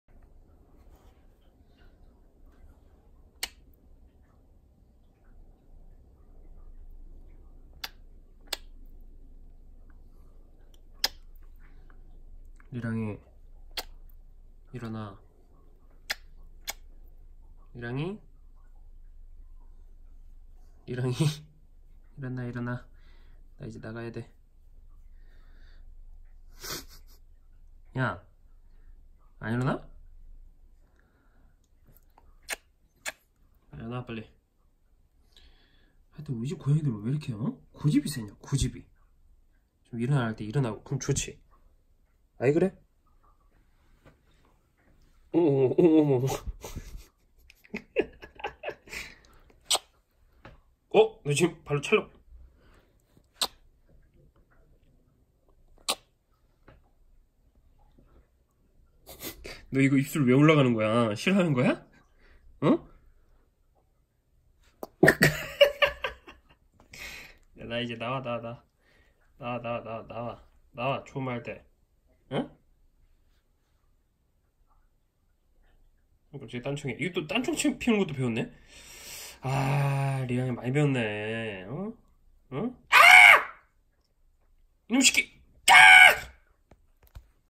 리랑이 뽀뽀로 때려서 깨워봤습니다.
상개냥이 리랑이는 제가 컴퓨터 혼자 하고 있는 꼴을 못 봅니다.

아 반대인가... 자기를 혼자 놔두는 꼴을 못 보는건가...